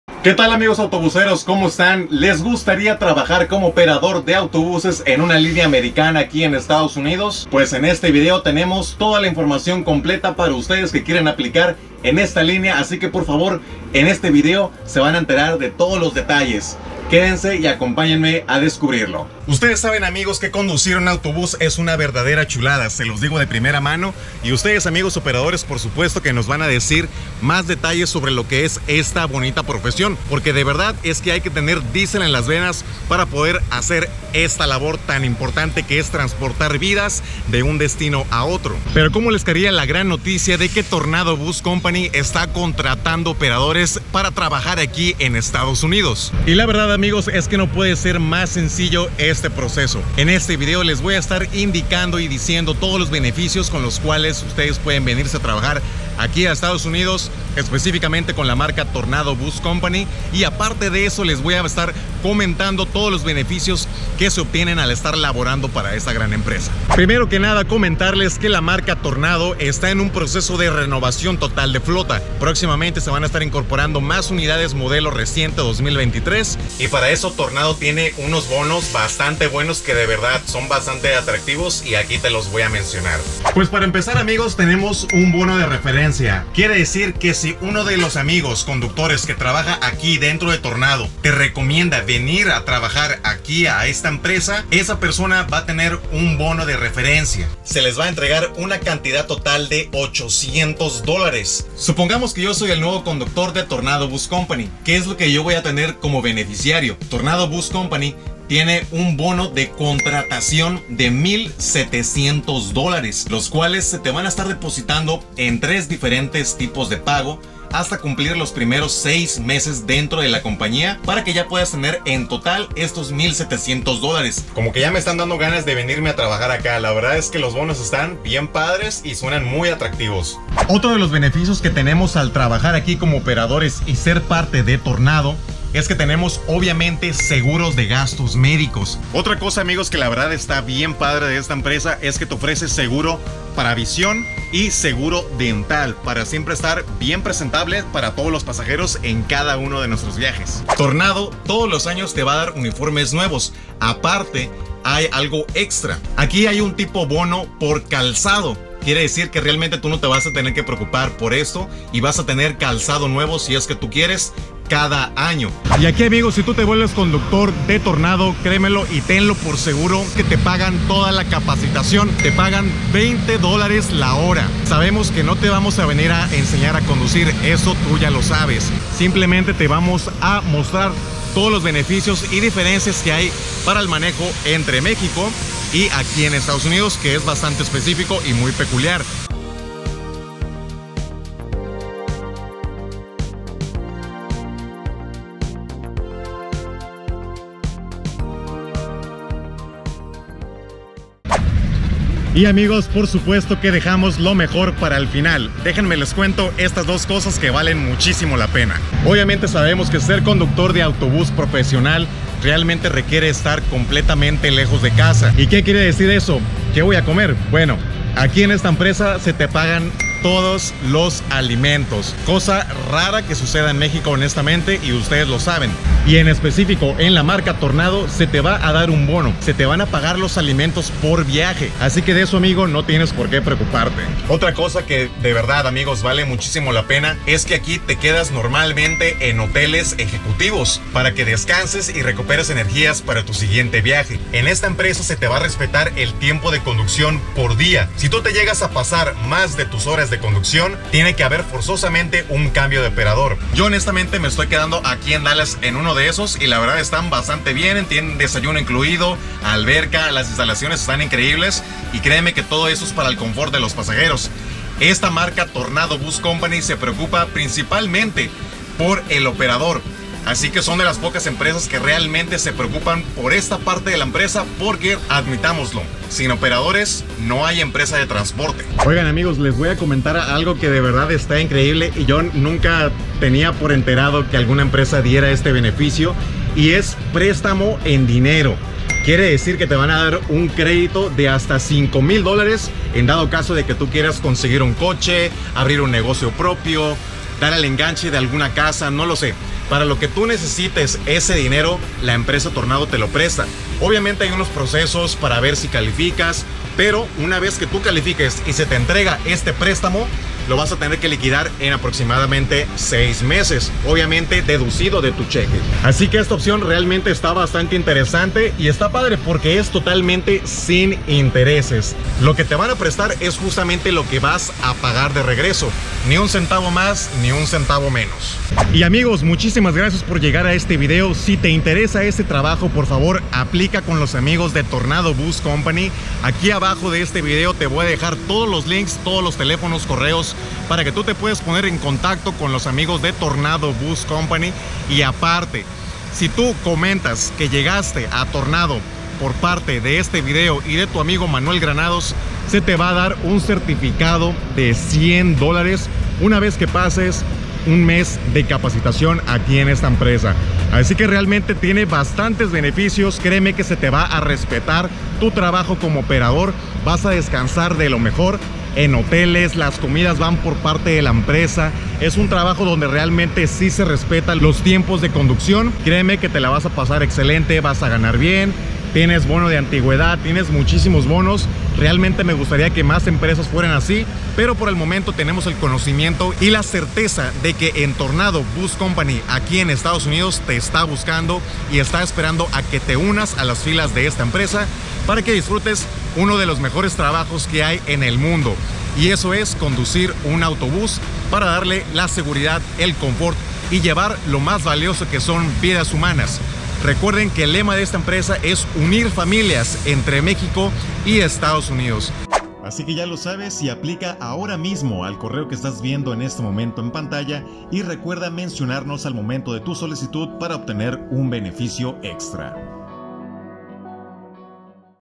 ¿CÓMO SER CONDUCTOR DE AUTOBÚS EN ESTADOS UNIDOS? | TORNADO BUS COMPANY
¿Quieres trabajar como operador de autobús en EEUU? TORNADO Bus Company te está buscando para que te unas a la empresa y en este vídeo te comento algunos de sus beneficios al trabajar para esta empresa!

RECUERDA MENCIONARNOS AL APLICAR PARA RECIBIR UN BONO EXTRA!!!